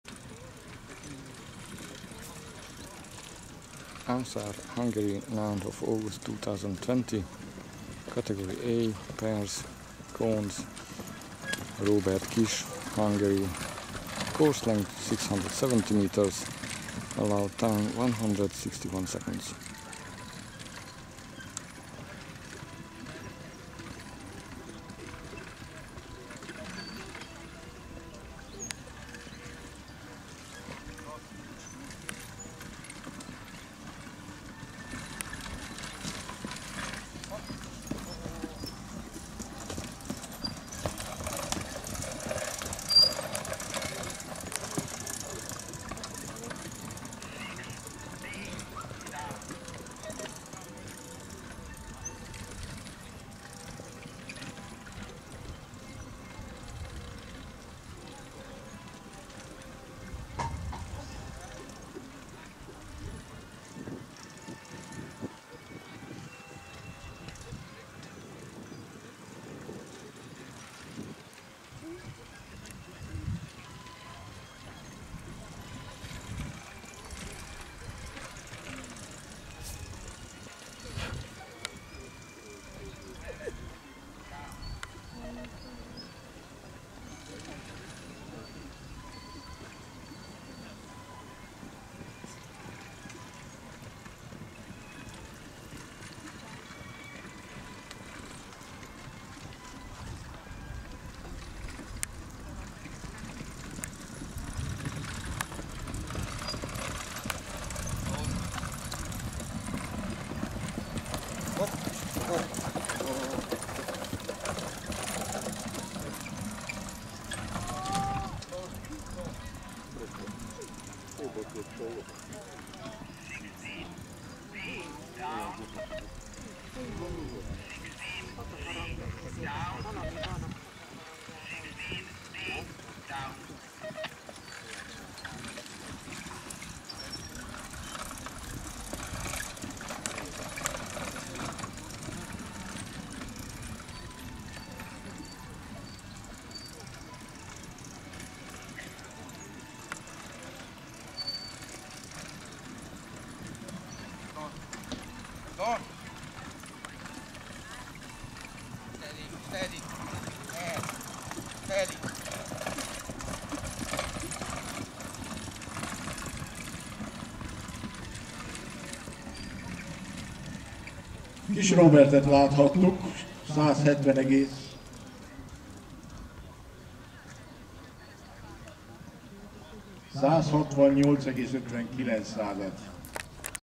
Róbert KISS (HUN) - CAI-A**, H2, Ászár (Hungary), cones, 09.08.2020
KISS Róbert (Magyarország) - CAI-A**, H2, Ászár (Magyarország), akadályhajtás (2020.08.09.)
lovak/horses: 17D: Stedi ??? - 17C: Sir Don ??? - 17E: Walter ??? FST
segédhajtó/groom: N/A ???
pálya hossza/course length: 670 m
alapidő/allowed time: 161 sec
168,59